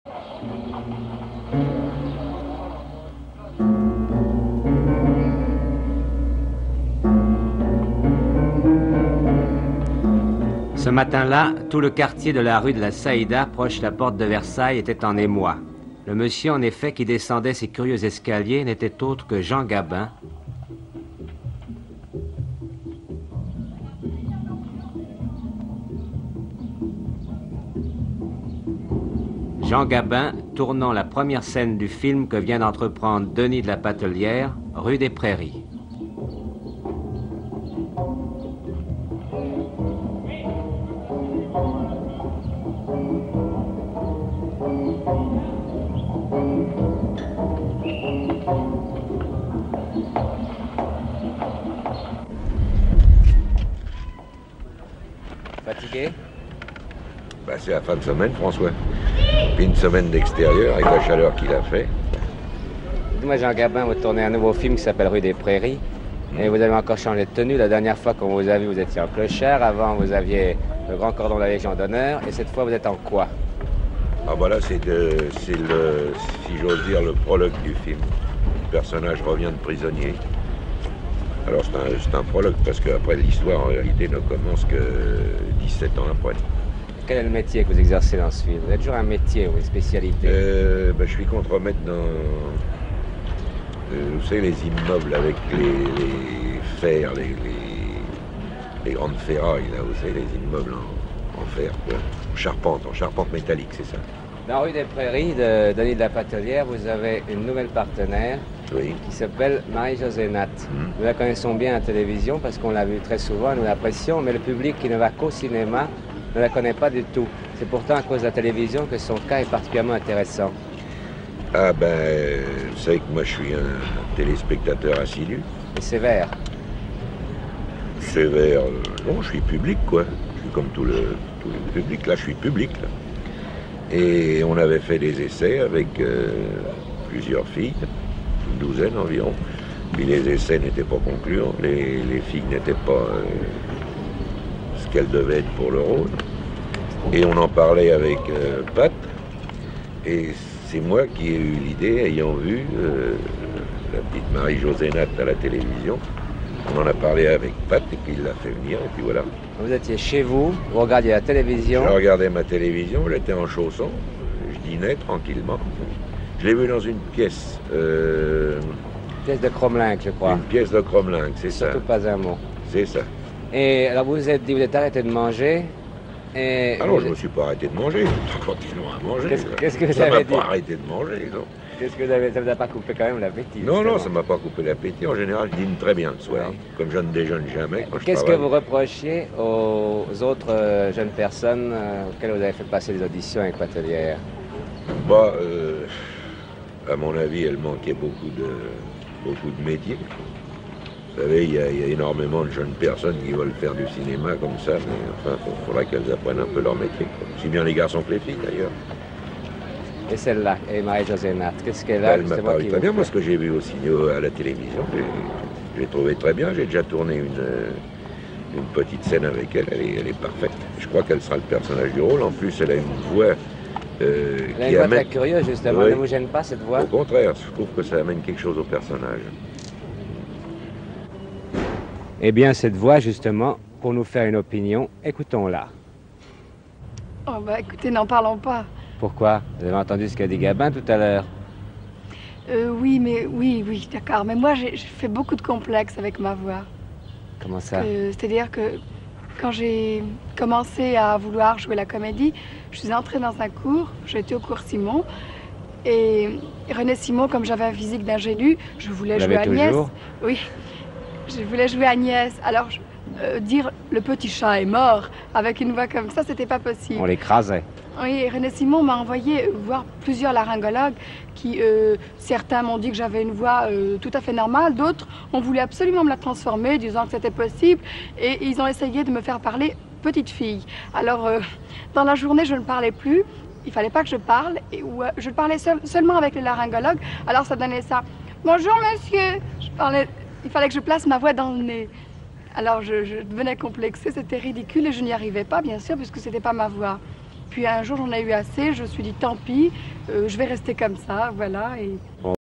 Jean Gabin et Marie-José Nat sur le tournage du film "Rue des prairies"
Jean Gabin et Marie-José Nat sur le tournage du film "Rue des prairies" de Denys de La Patellière, tourné aux Studios Eclair lors de l'été 1959.